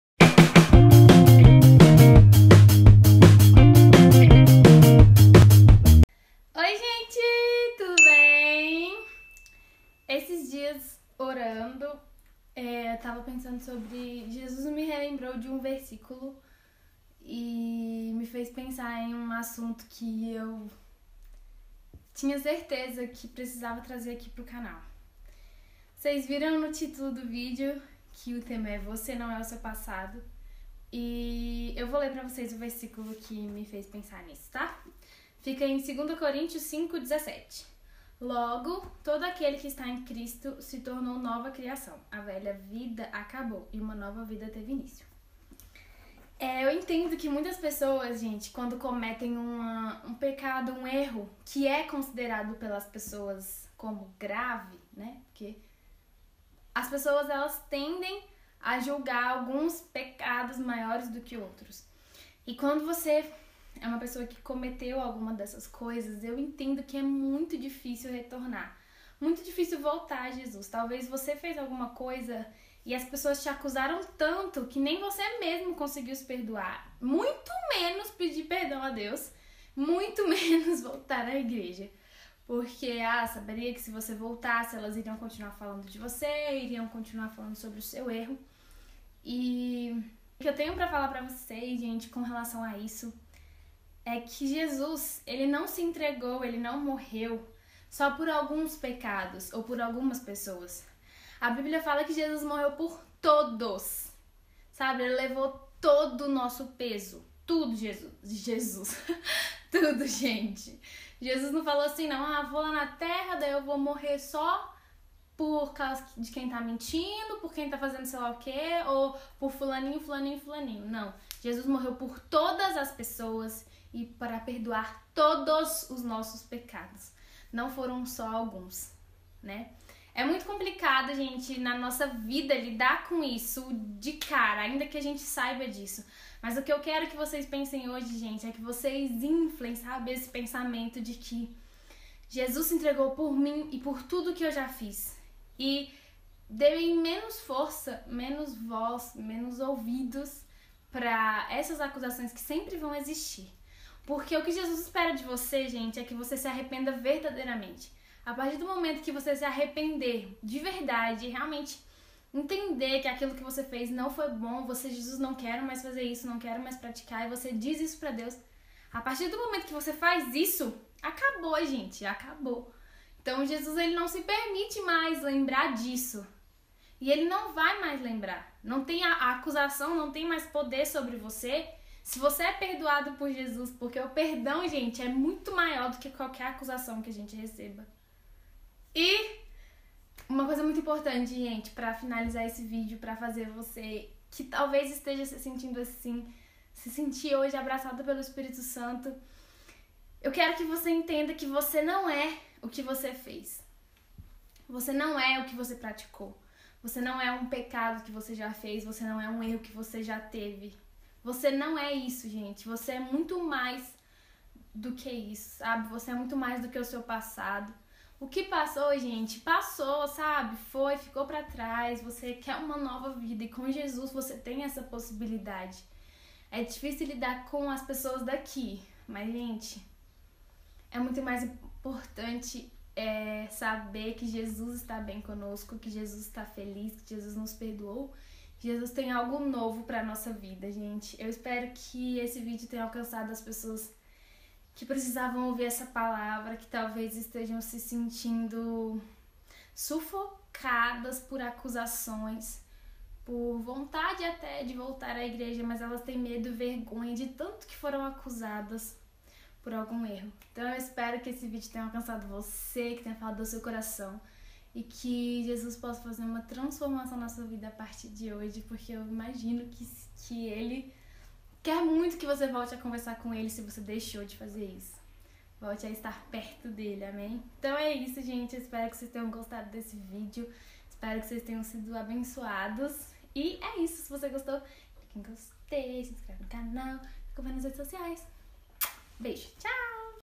VOCÊ NÃO É O SEU PASSADO!
Jesus tem uma vida nova para você, e se nem Ele próprio quer se lembrar dos seus pecados perdoados, porque continuar achando que o seu erro te definiu?! Vem ver!

Snap: thayna.linhares
Face: @blogdathaylinharess